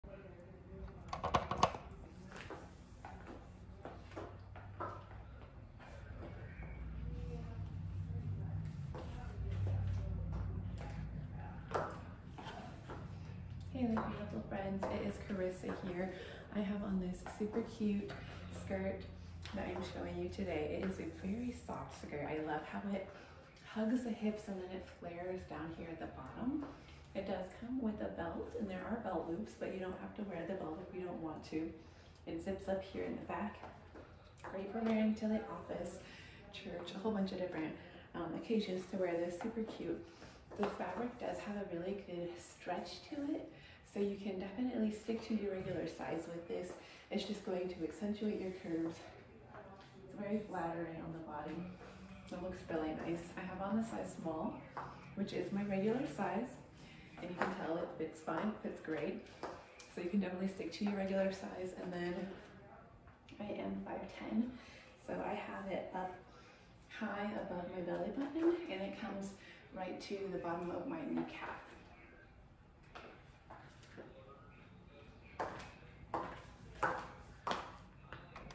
This skirt is so cute and perfect for the office
I love how this skirt fits and the style is so cute.
Wearing size small and it fits great